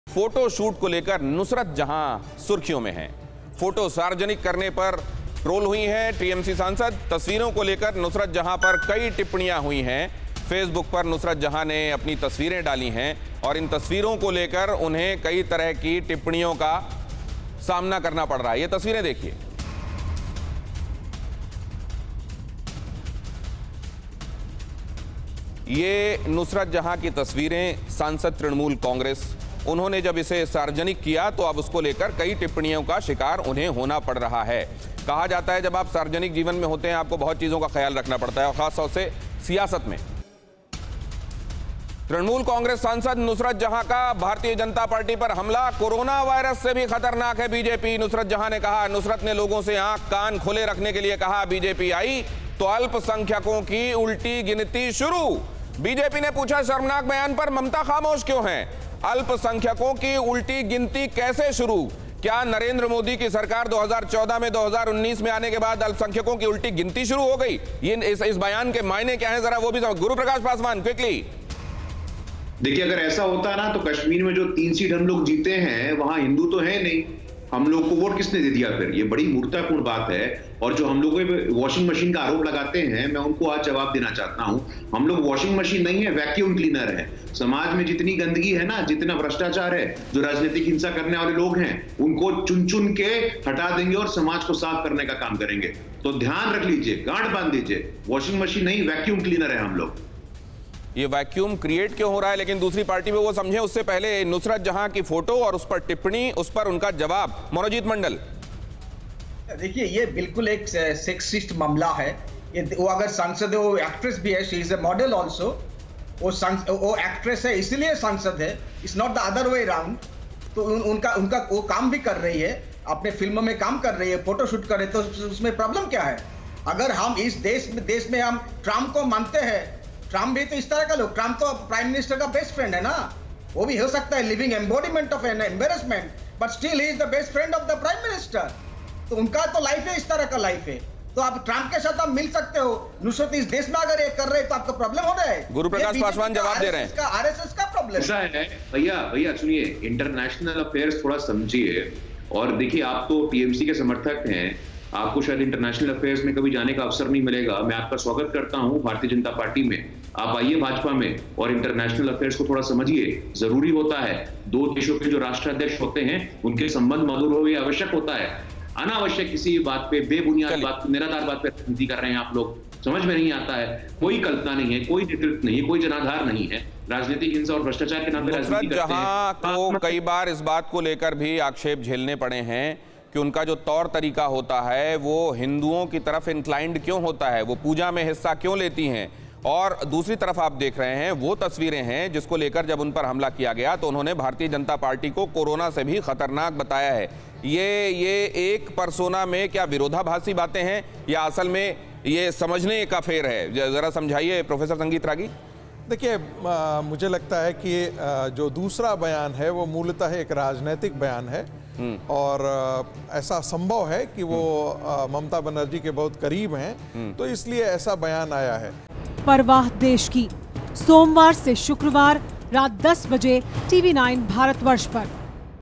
फोटो शूट को लेकर Nusrat Jahan सुर्खियों में, फोटो सार्वजनिक करने पर ट्रोल TMC सांसद | Adi
फोटो शूट को लेकर नुसरत जहां सुर्खियों में आ गई हैं. फोटो सार्वजनिक करने पर ट्रोल TMC सांसद, तस्वीरों को लेकर नुसरत जहां पर कई टिप्पणी भी की गई है. नुसरत जहां ने फेसबुक पर अपनी तस्वीर डाली है.